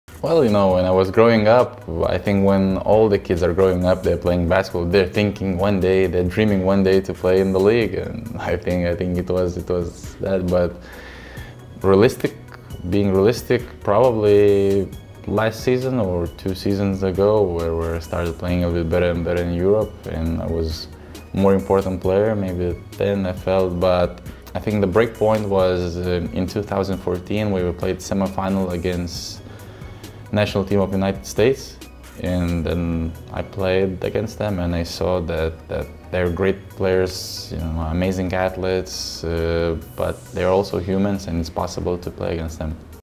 Knicks Stories: When did Kuz believe he could reach the NBA?
Watch to find out how Mindaugas Kuzminskas' childhood dream of playing in the NBA became a reality!

#️⃣ Twitter.com/nyknicks